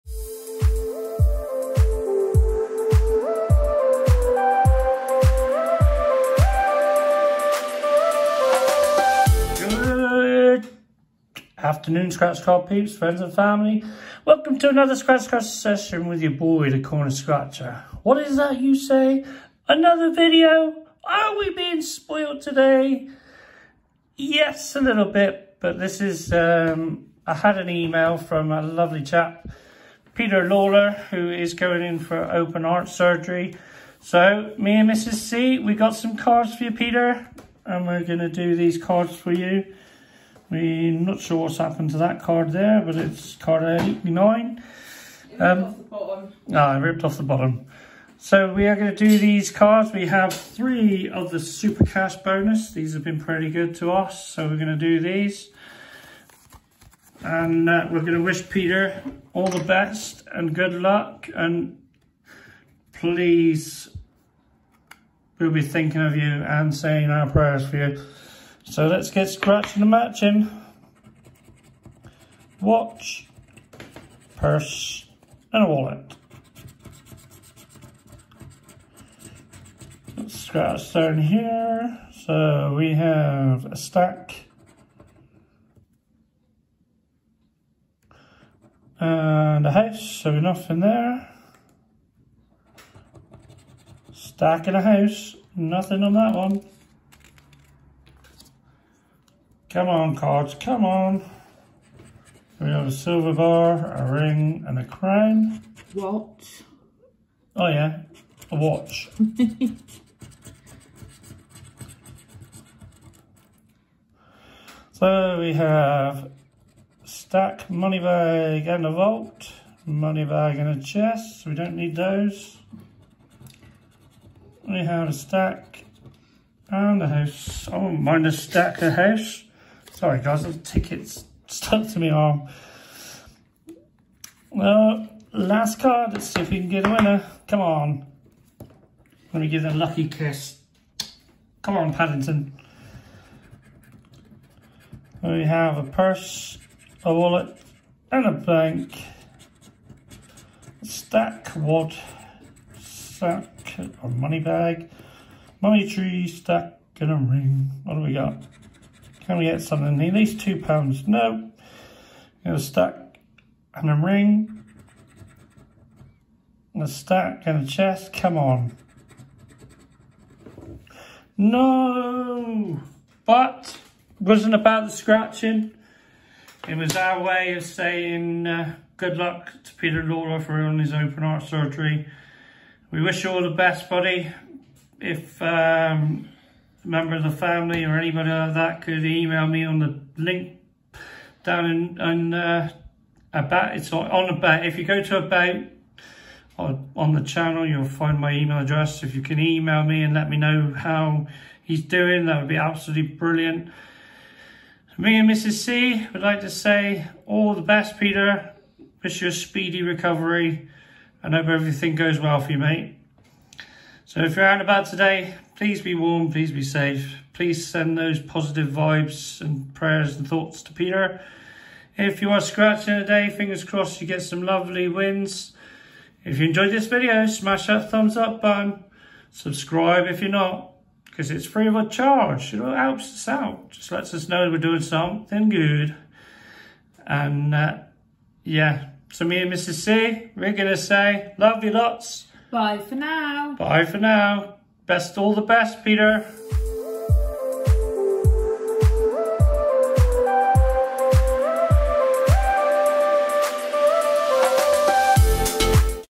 Peter Lawler we did these scratch cards for you and hope everything goes well in your operation
Peter emailed me to let me know that he’s going in for open heart surgery so we did some scratch cards for him and pray that hi open heart operation goes well and a speedy recovery
PLEASE SHARE WITH YOUR FAMILY AND FRIENDS